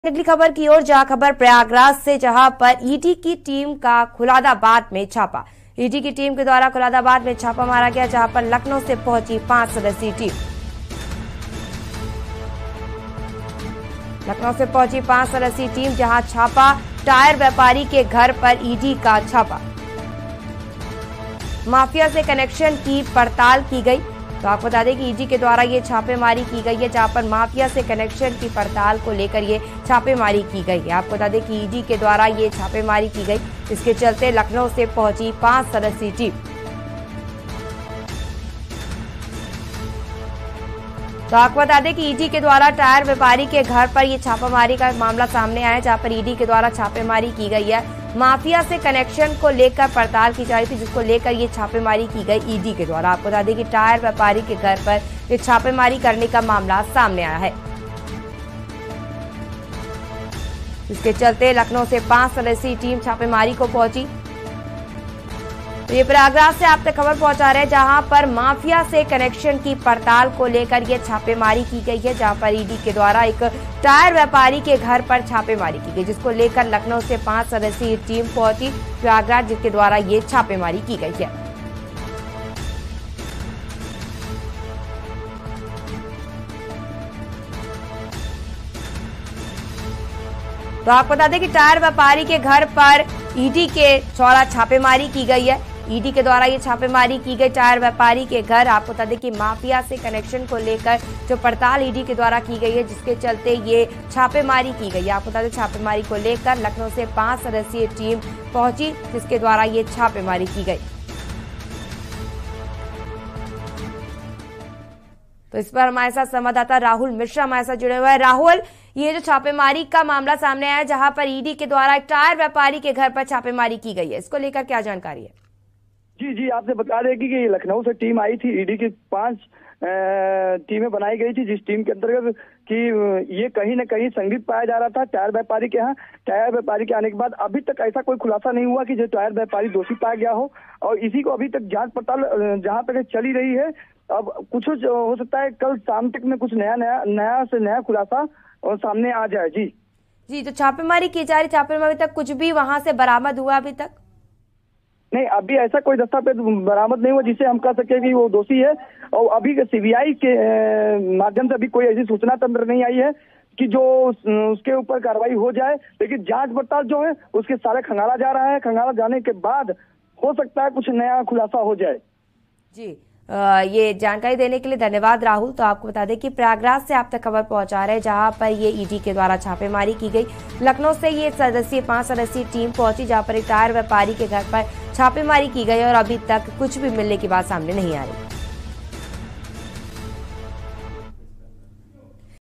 Prayagraj : ED की टीम का खुल्दाबाद में छापा, टायर व्यापारी के घर पर ED का छापा || K News India
ED की टीम का खुल्दाबाद में छापा. लखनऊ से पहुंची 5 सदस्यीय टीम. टायर व्यापारी के घर पर ED का छापा. माफिया से कनेक्शन की पड़ताल की गई.